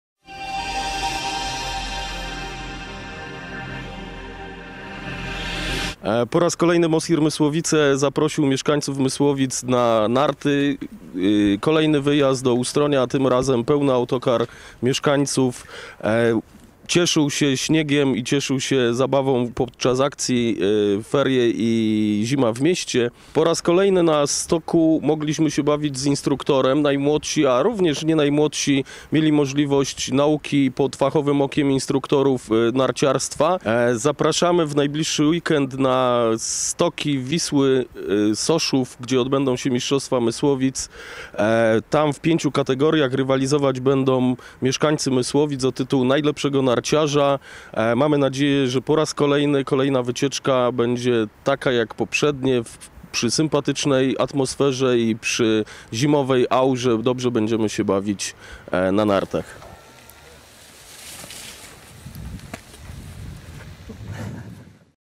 Akcja Zima w mieście na Sportowo: Wyjazd na narty.
10 lutego odbył się kolejny wyjazd na narty organizowany przez Miejski Ośrodek sportu i Rekreacji w ramach akcji "Zima w mieście na sportowo". Tym razem mieszkańcy Mysłowic mieli okazję poszusować na nartach oraz snowboardzie w Ustroniu na stokach Wielkiej Czantorii. Nad bezpieczeństwem najmłodszych miłośników zimowych sportów czuwał instruktor narciarstwa, który udzielał cennych porad oraz wskazówek. Mimo że pogoda nie dopisała humory były wyśmienite.